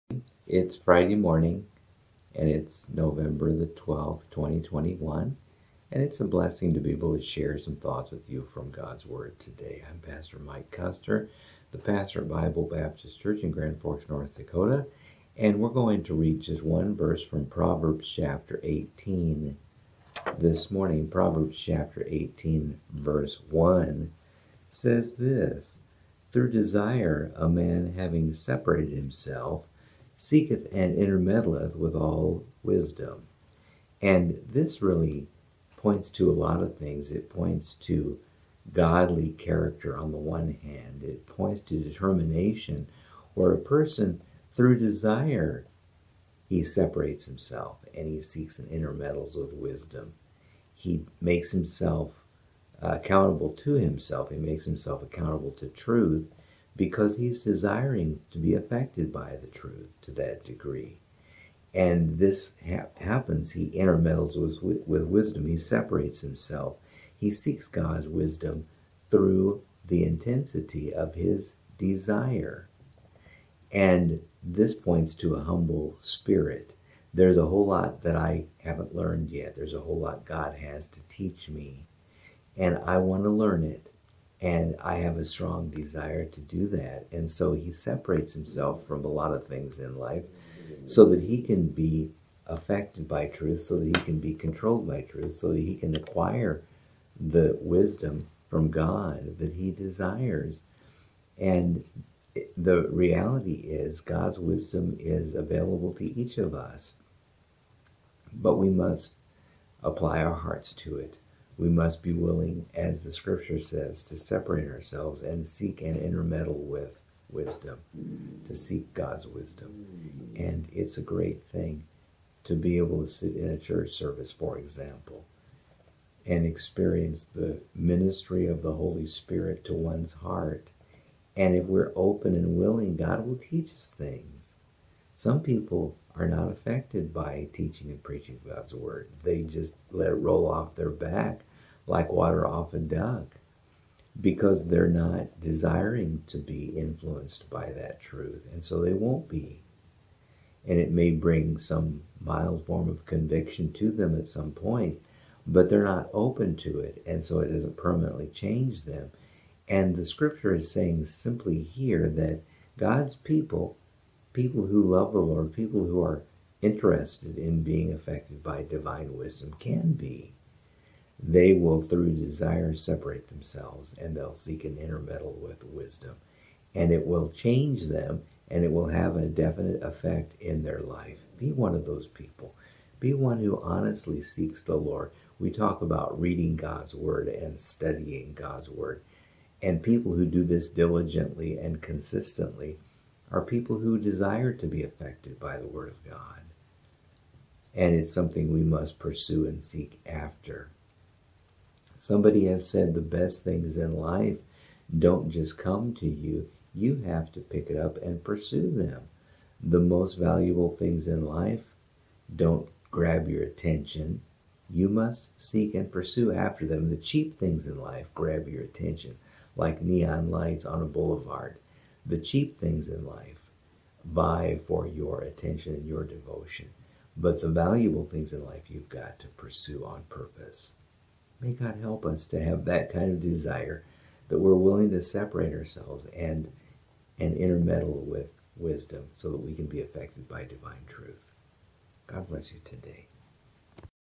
Daily Devotions - Affected by Divine Truth (Nov. 12, 2021)
Proverbs 18:1 Through desire a man, having separated himself, seeketh and intermeddleth with all wisdom.